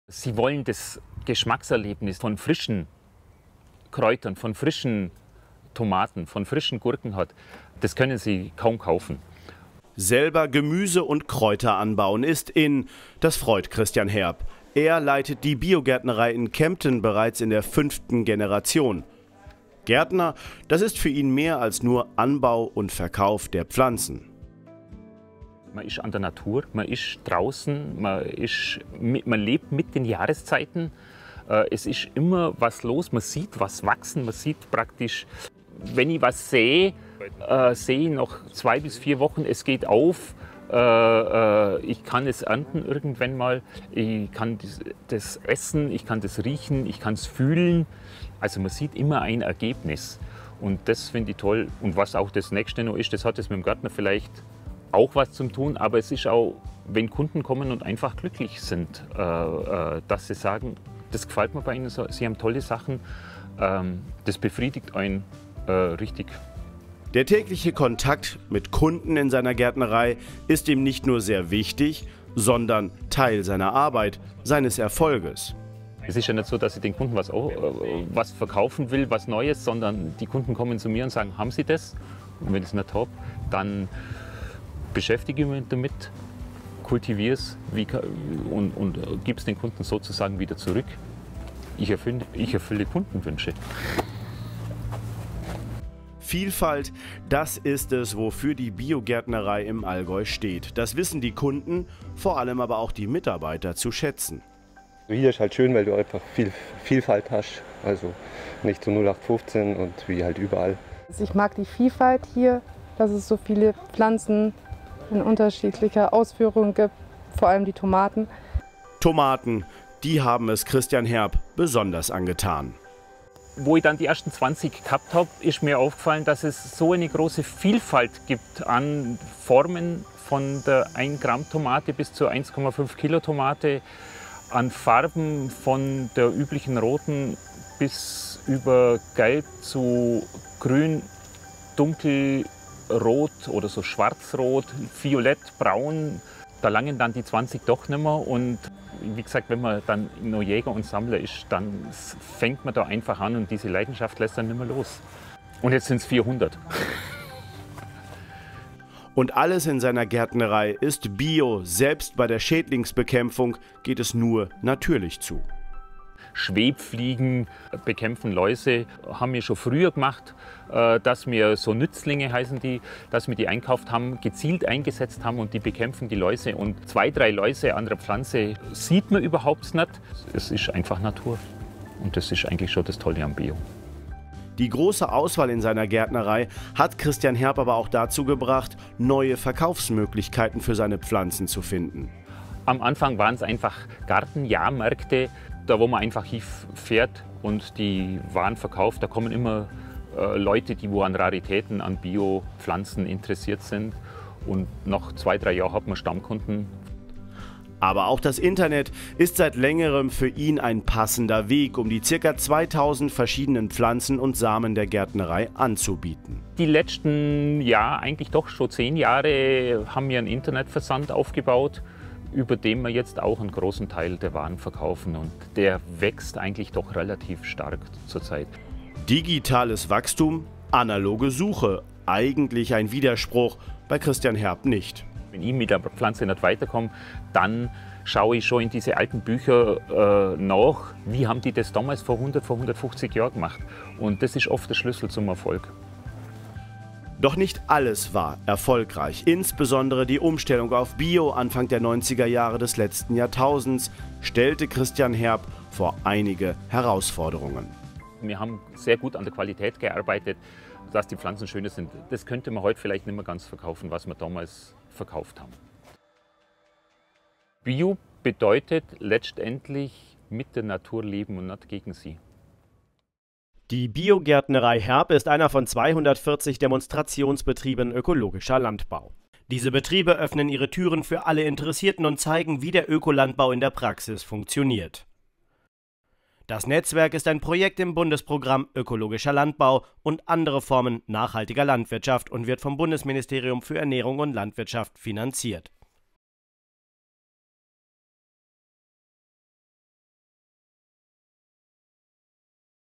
Biogärtnerei Herb: "Bunte (Tomaten-)Vielfalt"
Anfang der 1990er Jahre stellte der Allgäuer Christian Herb seine Kemptener Gärtnerei auf Bio um. Die größte Herausforderung dabei: den Ansprüchen der Verbraucherinnen und Verbraucher an formschönes Gemüse zu genügen. Heute steht bei seinen Kundinnen und Kunden etwas anderes im Vordergrund: das Geschmackserlebnis, Frische und der Do-It-Yourself-Gedanke. Vielfalt ist sowohl für den Demonstrationsbetrieb als auch für alle Hobbygärtnerinnen und -gärtner das entscheidende Stichwort. Im Laufe der Jahre hat der selbsternannte „Jäger und Sammler“ Herb sein Angebot von 20 auf 400 Tomatensorten erweitert. Die besonderen Formen, Farben und Geschmacksrichtungen kommen gut an: „Es werden immer wieder seltene, besondere Sorten nachgefragt. Dann recherchiere und kultiviere ich sie. So erfülle ich Kundenwünsche und das macht mich sehr zufrieden“, berichtet Herb. Weitere Erfolgsrezepte der Naturland-Gärtnerei verrät das neueste demoVIDEO der Reihe „Raus aufs Land“.